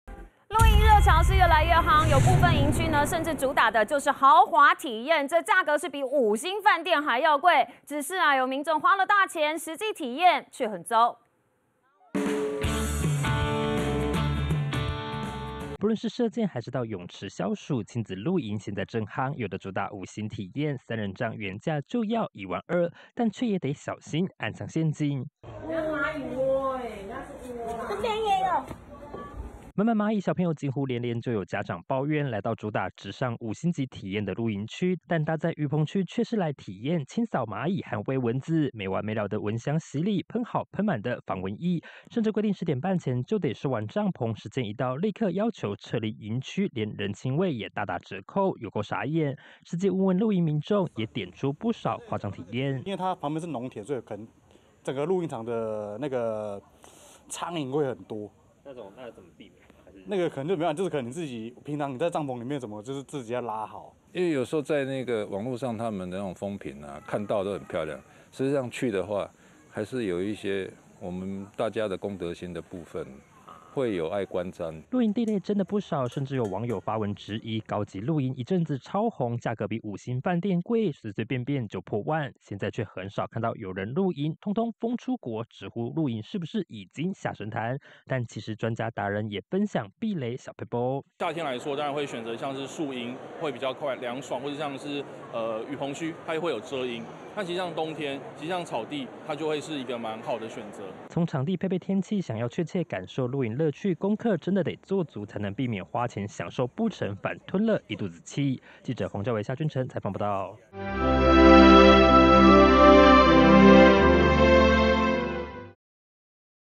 比五星飯店貴！ 豪華露營「掃螞蟻、餵蚊」受一肚氣
國人近年來熱愛露營，有些營區更主打豪華體驗，價格比五星飯店還要貴，卻有民眾實際前往，怒轟體驗到的全是清掃螞蟻和餵蚊子。實際詢問許多露營營主，還真的有不少誇張體驗，難怪有網友直呼露營似乎跌下神壇，不如出國玩來的過癮。

下載【年代電視】APP：
iOS平台：